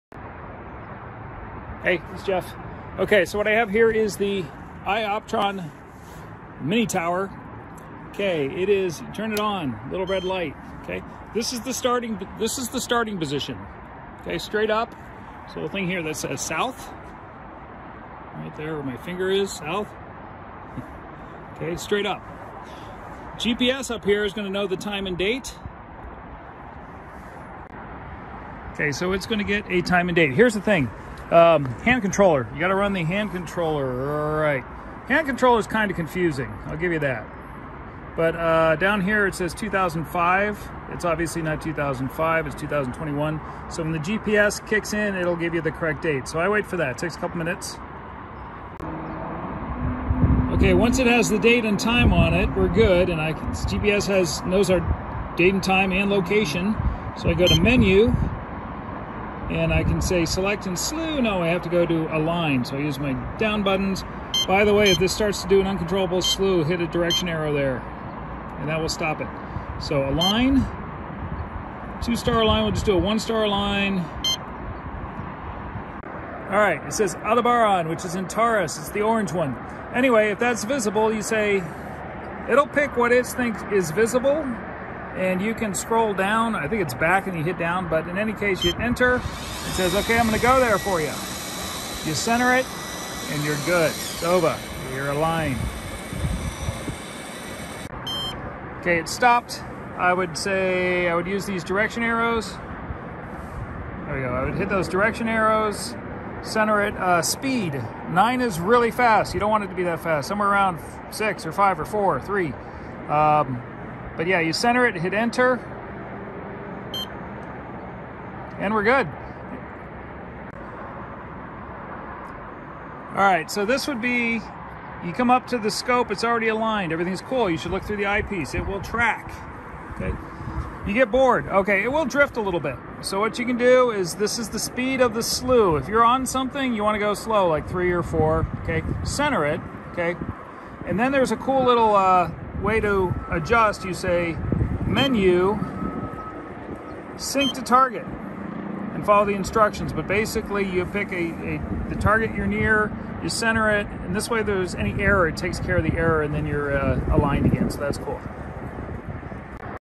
iOptron MiniTower II - 2.5 minutes to Align and Find
The iOptron MiniTower II is the easier mount I've ever used to Align to the Sky and Start Using.  It's not perfect, but it is FAST and EASY to Align.

Here's how to Align and finding objects in 150 seconds.

Note: you'll want to to do a little more, though.  For example, I skipped leveling the mount head and didn't talk about precautions (the Altitude Clutch has a bad habit of breaking loose:  make sure it's tight, make sure all your accessories are on tight in case it does break loose).